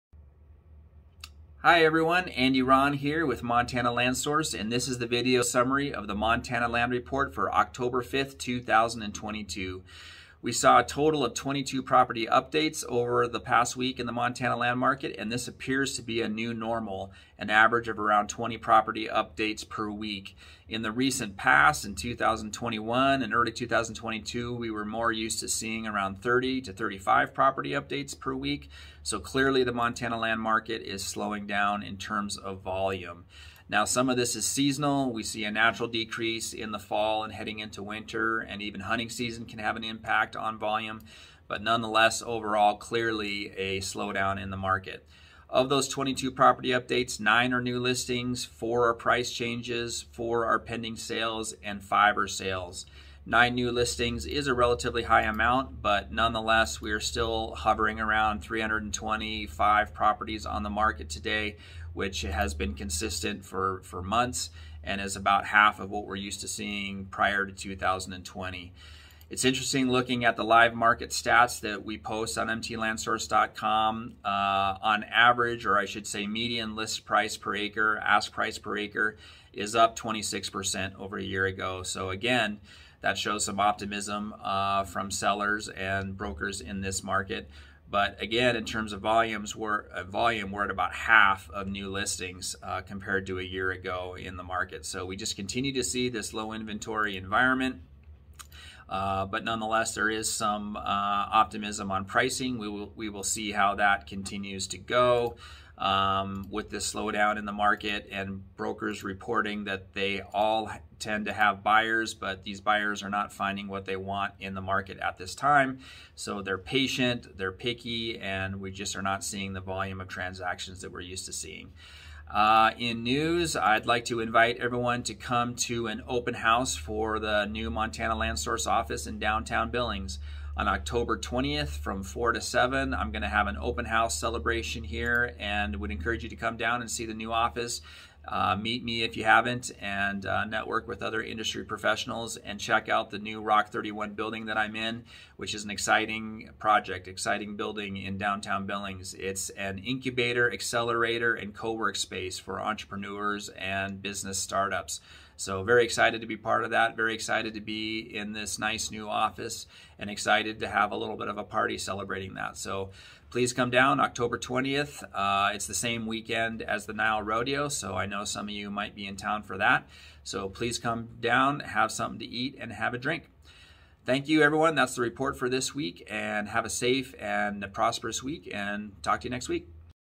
Montana Land Report 10/5/2022
This is a brief video summary of the weekly Montana Land Report from Montana Land Source for the week of 10/5/2022.  The Montana Land Report summarizes the week's activity in the Montana land market, as well as Montana land news and events, and goes out to over 5,500 recipients.  The Montana Land Report is free and available